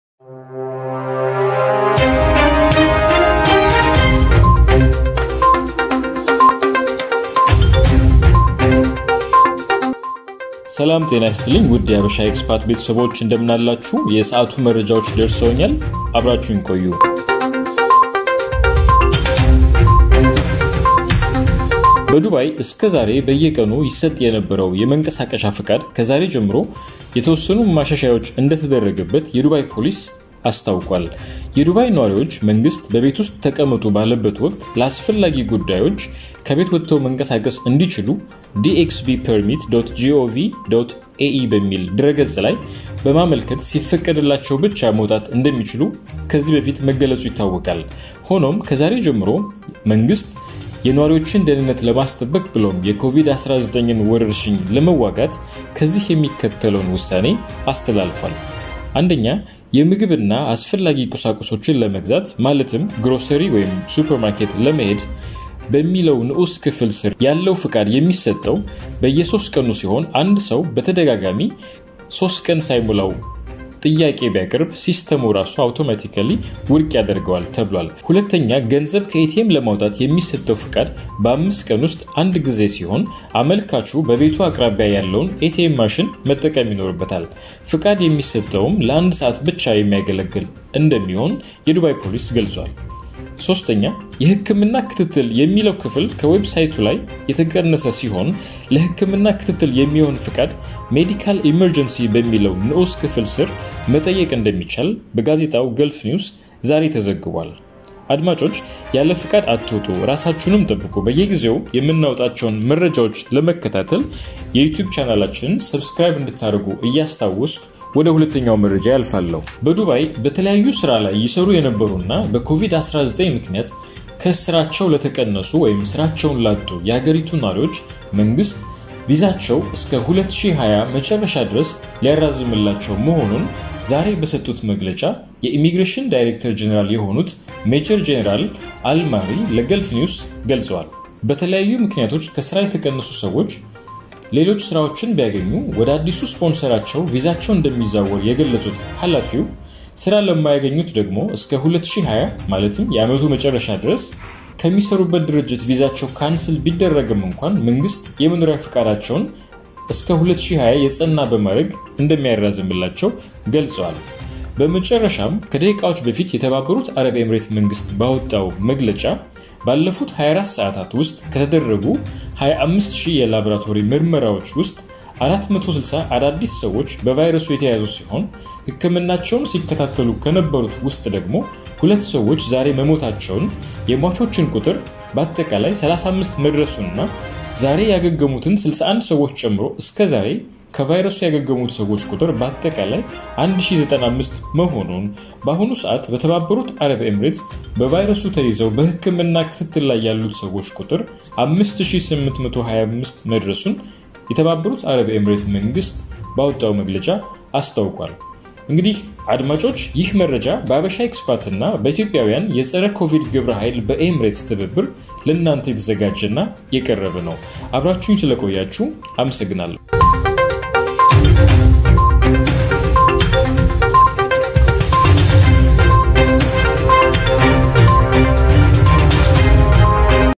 Ethiopia በዱባይ ከስራ ለተቀነሱ ሰዎች መንግስት ቪዛቸውን እስከ ዓመቱ መጨረሻ ሊያራዝም ነው
ሐበሻ ኤክስፓት በመላው ዓለም ያለውን የሐበሻ ህብረተሰብ ተአማኒነት ያለው  ትኩስ ዜናዎችን ፤ የመዝናኛ ፕሮግራሞችንና ሙዚቃዎችን ለማድረስ ታስቦ የተዘጋጀ መድረክ ነው::